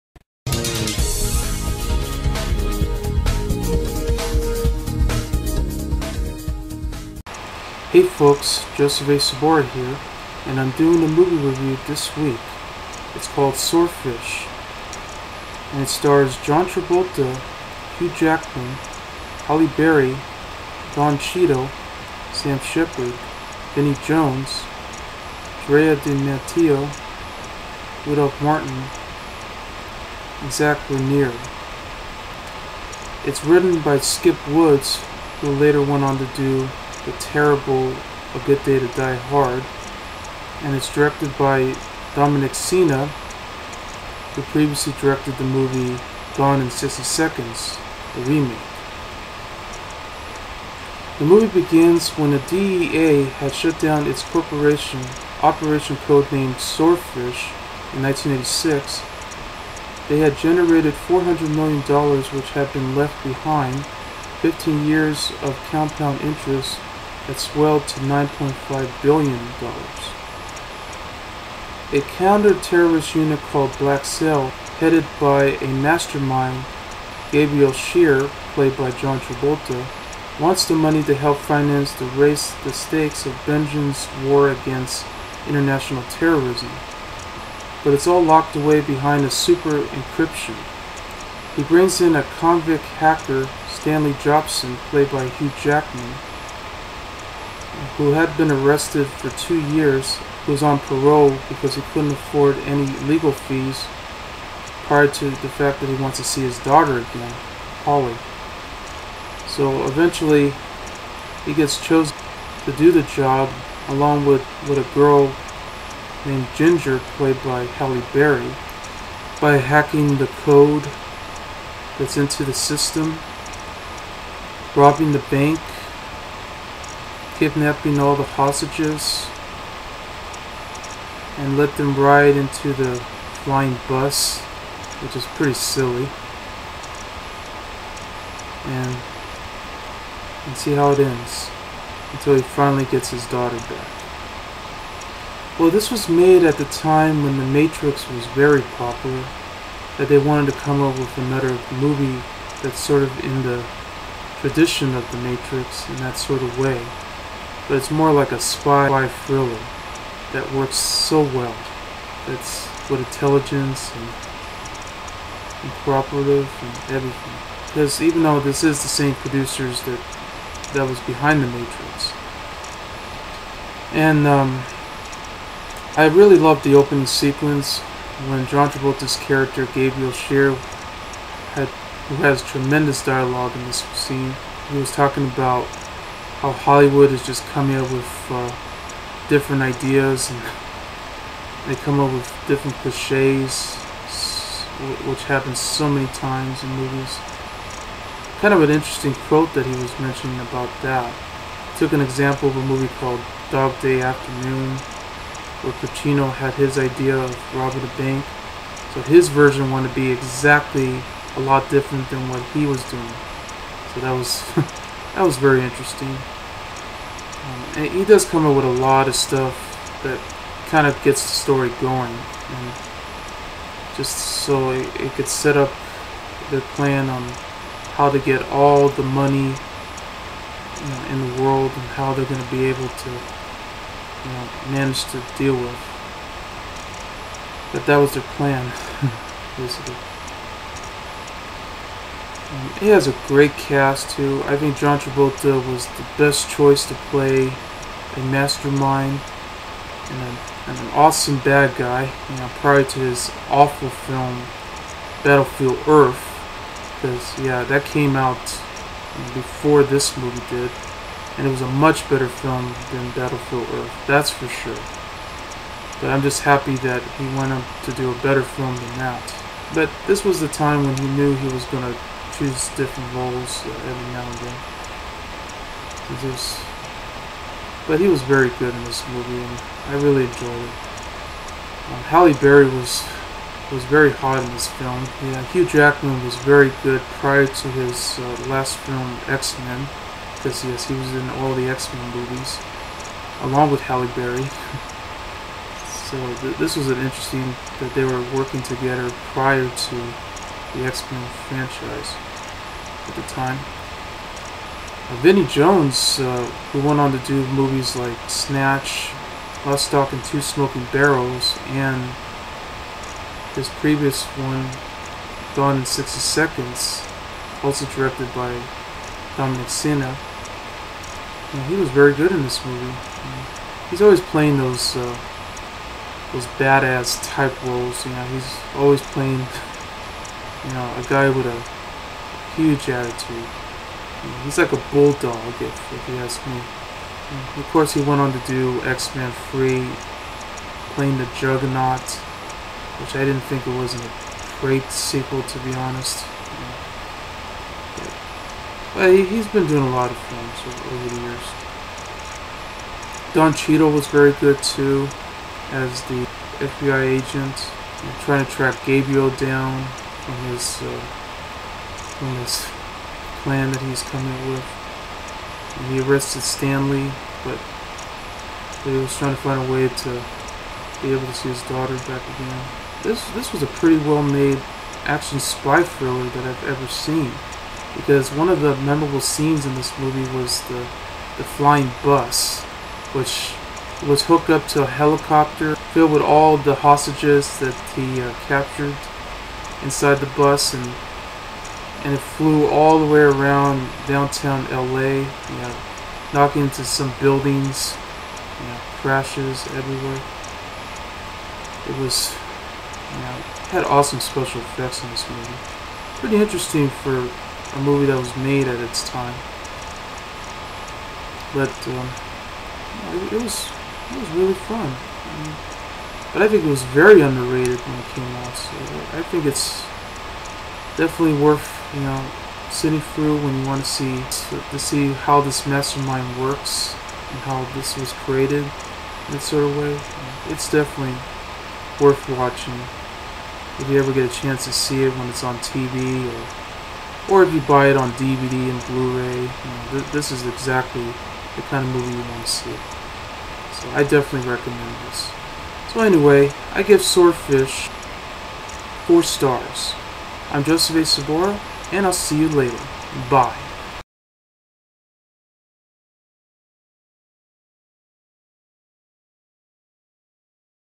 Swordfish (2001): Joseph A. Sobora's Movie Review
My Movie Review of "Swordfish" (2001).
Starring: John Travolta, Hugh Jackman, Halle Berry, Don Cheadle, Vinnie Jones, Sam Shepard, Drea de Matteo, Rudolf Martin, Zach Grenier & Camryn Grimes.

Written By: Skip Woods (A Good Day to Die Hard). Directed By: Dominic Sena (Gone in 60 Seconds).